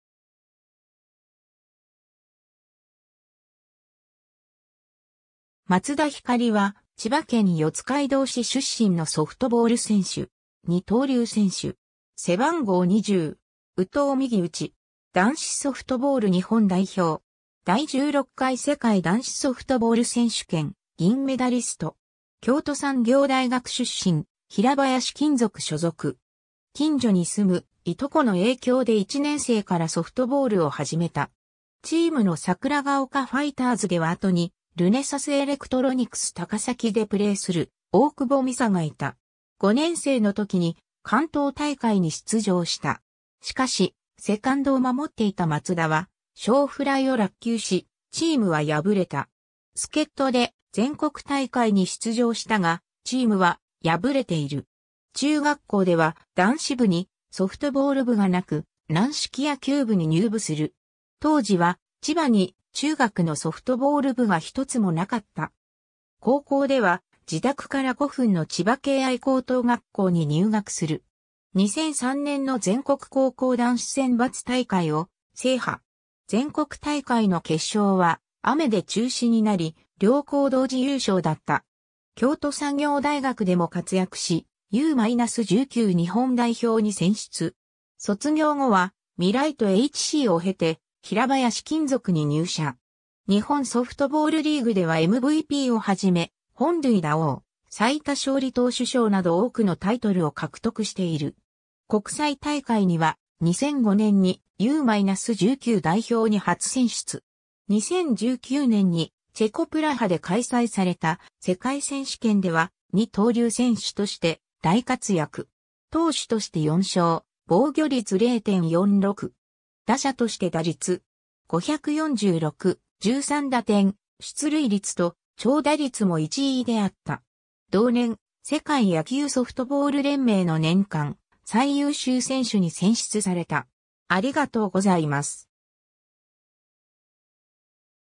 松田光
松田 光（まつだ ひかる、1986年12月17日 - ）は、千葉県四街道市出身のソフトボール選手（投手）。
二刀流選手。
背番号20。
右投右打。
男子ソフトボール日本代表。
 第16回世界男子ソフトボール選手権銀メダリスト。
京都産業大学出身、平林金属所属（主将）。
 近所に住む従兄弟の影響で1年生からソフトボールを始めた。
チームの桜ヶ丘ファイターズでは後にルネサスエレクトロニクス高崎でプレーする大久保美紗がいた。
5年生の時に関東大会に出場した。
しかし、セカンドを守っていた松田は小フライを落球し、チームは敗れた。
助っ人で全国大会に出場したが、チームは敗れている。
 中学校では男子部にソフトボール部がなく、軟式野球部に入部する。
当時は千葉に中学のソフトボール部が一つもなかった。
 高校では自宅から５分の千葉敬愛高等学校に入学する。
2003年の全国高校男子選抜大会を制覇。
全国大会の決勝は雨で中止になり両校同時優勝だった。
京都産業大学でも活躍し、U-19日本代表に選出。
卒業後は、未来都HCを経て、平林金属に入社。
 日本ソフトボールリーグではMVPを始め、本塁打王、最多勝利投手賞など多くのタイトルを獲得している。
 国際大会には、2005年にU-19代表に初選出。
 2019年にチェコ・プラハで開催された世界選手権では、二刀流選手として大活躍。
投手として4勝、防御率0.46。
打者として打率.546、13打点、出塁率と長打率も1位であった。
 同年、世界野球ソフトボール連盟 (WBSC)の年間最優秀選手に選出された。